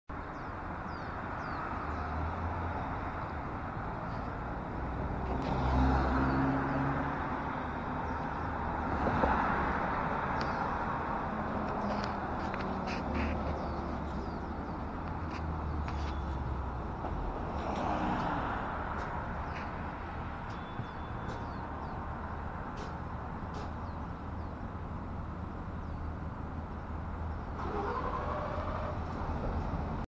outdoors - dine in eateries in orange county,ca today
how does dining in an enclosed outdoor tent is more flying bugs free than dining inside a nice upscale dining restaurant? similar foods and ingredients, utensils, chefs, waitress/waitors, monetary payments, etc.. huh...